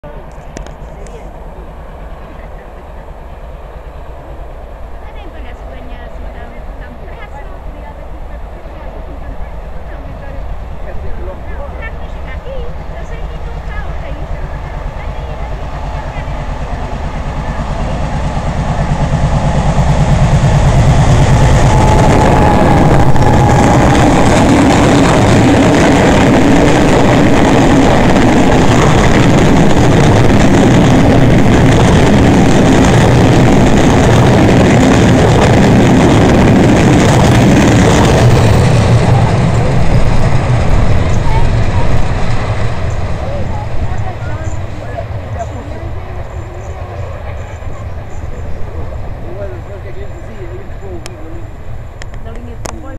CP CARGA»BOMBARDIER 1971 EM PATAIAS
Ramalhal» Mangualde com cereais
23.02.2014
17,25h